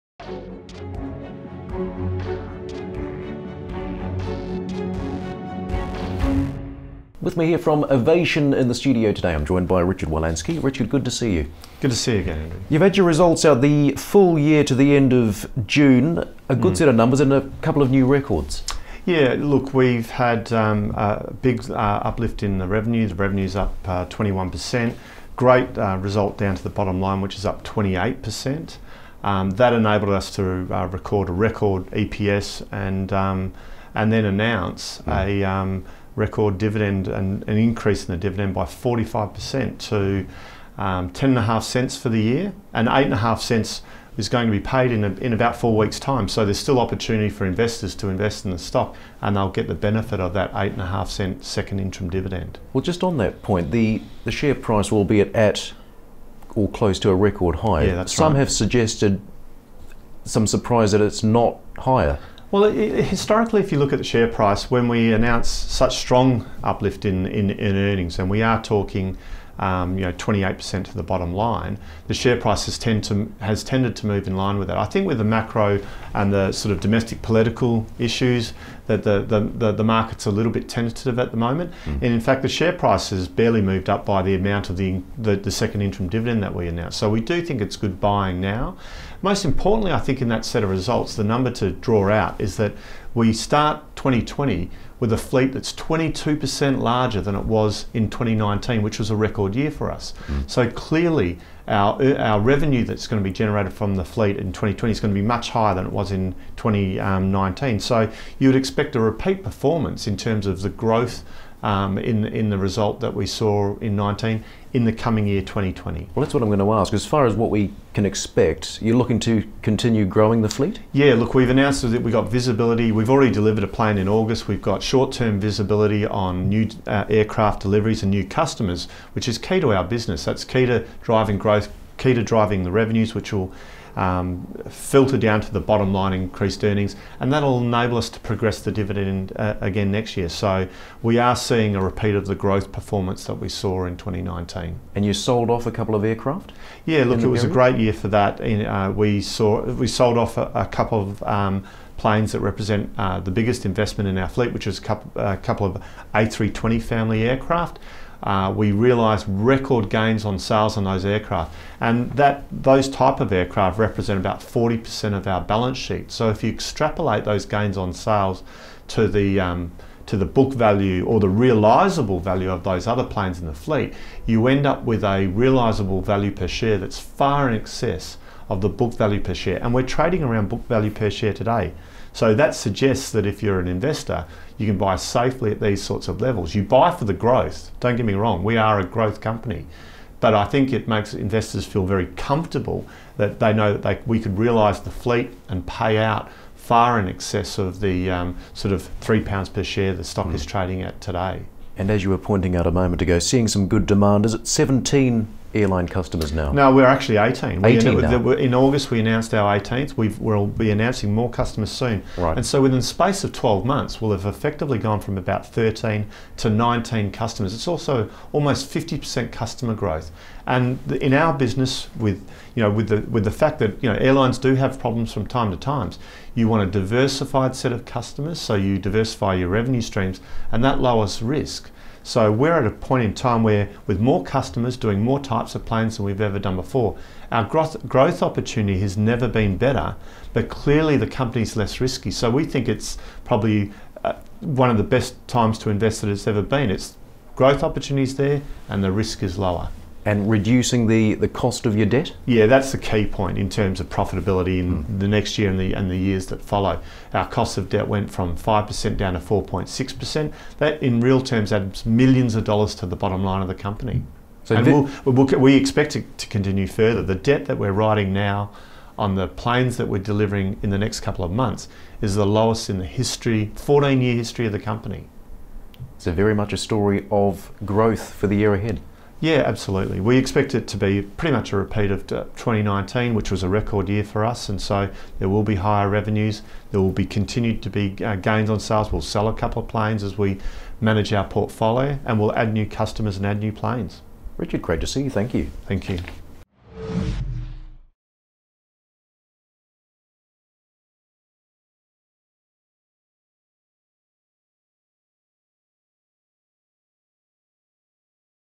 Avation PLC soaring as it reports record revenue, profit and fleet assets
Avation PLC's (LON:AVAP) Richard Wolanski caught up with Proactive London's Andrew Scott following the release of the aircraft leasing company's full year results to June 2019.

The firm achieved both record revenues and profits and investors were rewarded with a bumper dividend pay-out too.